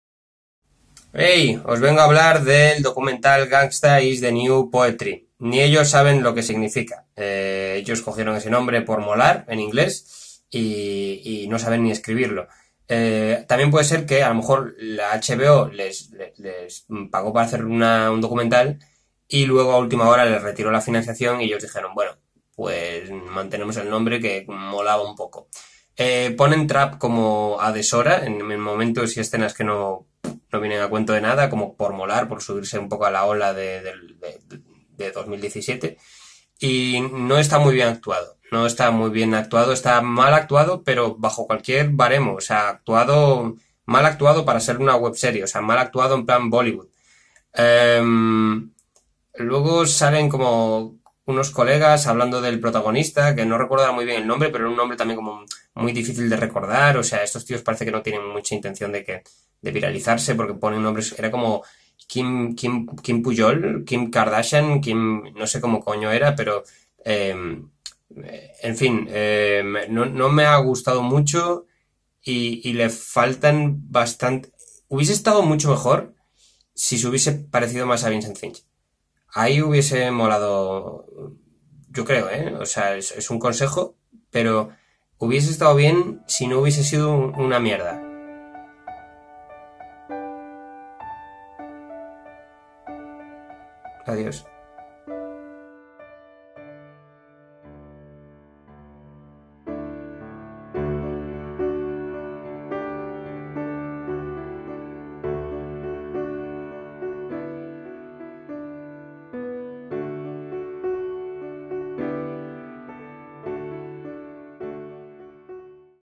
David Suárez se caga en el docu
Se filtra un vídeo del creador de Vincent Finch, David Suárez, y nos da la del atún, loco.

25 JULY

TW: @gangstapoetry69
FB: Gangsta is the new Poetry

Gangsta is the new Poetry 2017 ©